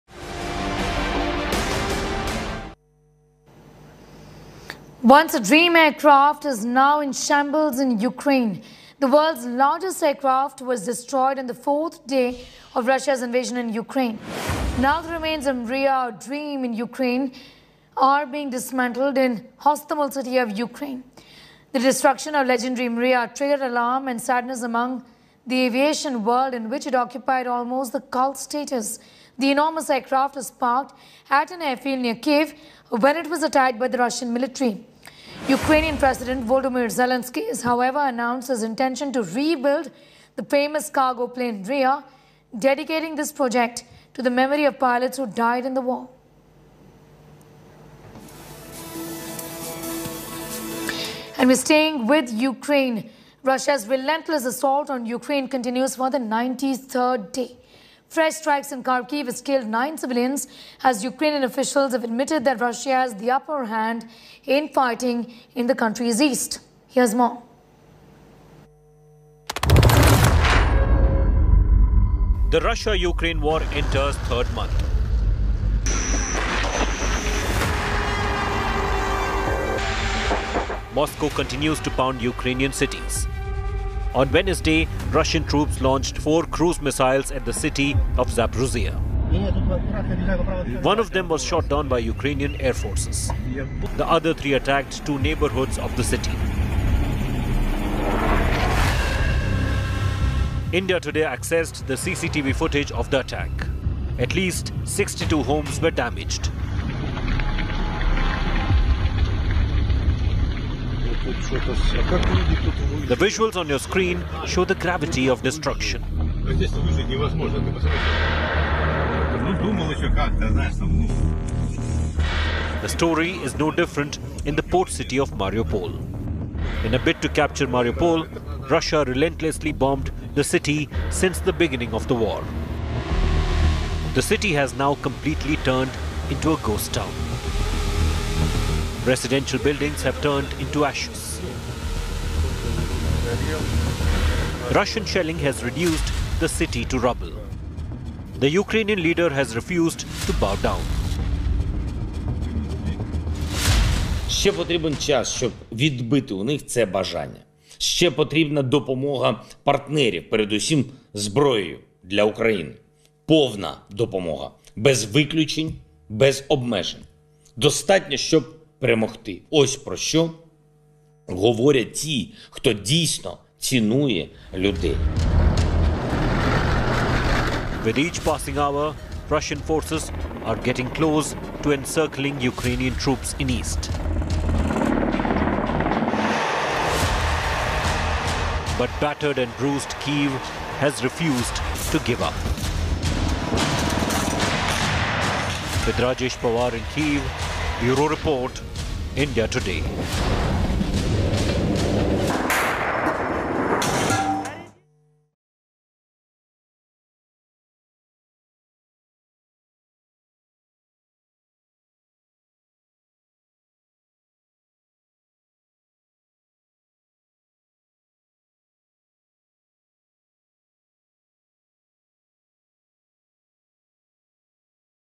'Mriya' Aircraft Pride Of Ukraine Aviation In Shambles, Zelenskyy Assures To Rebuild 'Mriya'
Once the dream aircraft has now shambled in Ukraine, the world's largest aircraft was destroyed on the 4th day of Russia's invasion in Ukraine. Now the remains of 'Mriya' dreams in Ukraine are being dismantled in Hostomel city of Ukraine. Ukrainian President Volodymyr Zelensky has announced his intention to rebuild the famous 'Mriya' dedicating this project to the memory of pilots who died in the war.